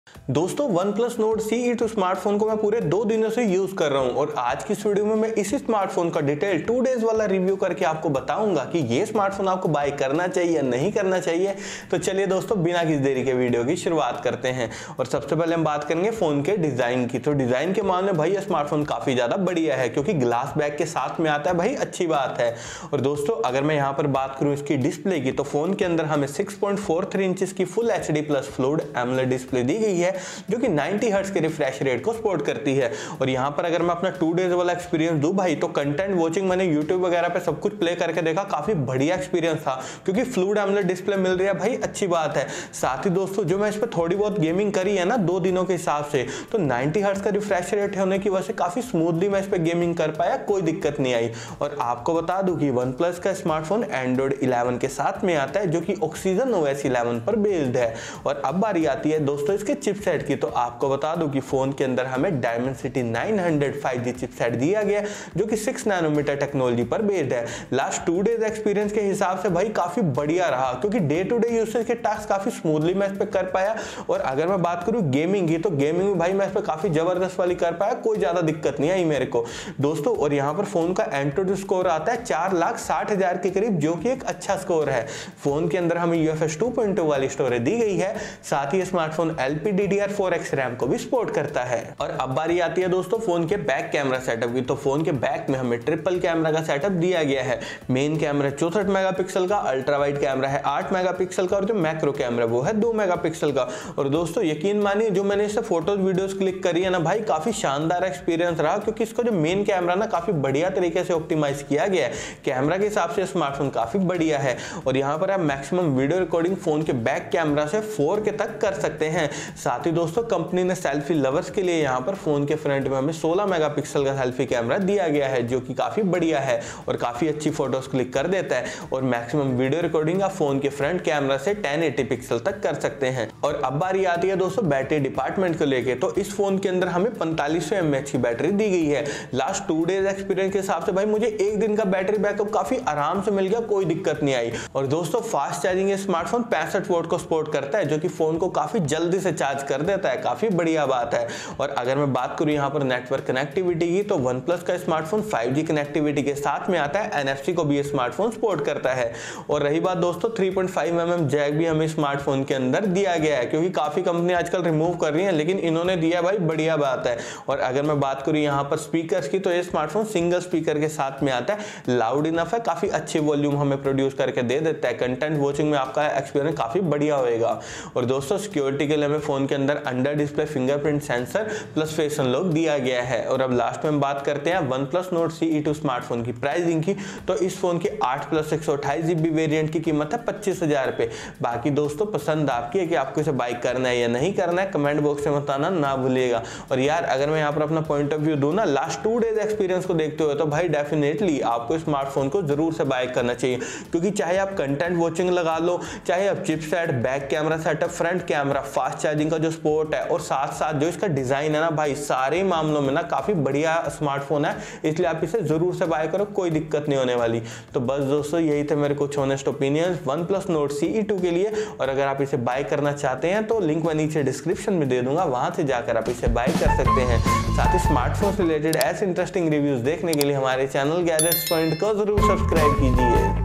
OnePlus Nord CE2 5G After 2 days Honest Review | Design + Battery + Specs | GALTI MAT KARNA ??
OnePlus Nord CE2 5G After 2 days Honest Review | Design + Battery + Specs | OnePlusNordCE25GReview2Days | GALTI MAT KARNA ??

Dosto, Aaj ki is video mey hum Honest & Detailed Review karenge, OnePlus Nord CE2 5G Smartphone ka.. Jisme hum aapko bataenge, ke aapko is phone ko buy karna chahiye ya nahin karna chahiye !!!

Topics Cover :-

1. OnePlus Nord CE2 5G after 2 days review
2. OnePlus Nord CE2 5G 2 days review